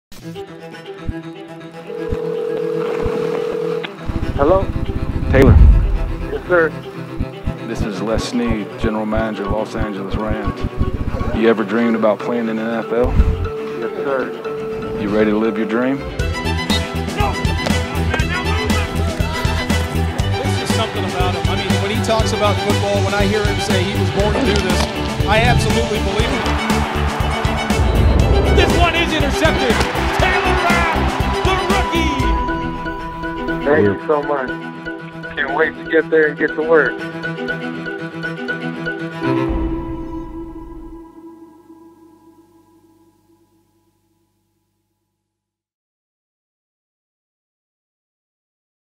"You Ready to Live Your Dream?" Taylor Rapp's Journey to the NFL
In the second round of the 2019 NFL Draft, the Rams selected safety Taylor Rapp out of the University of Washington.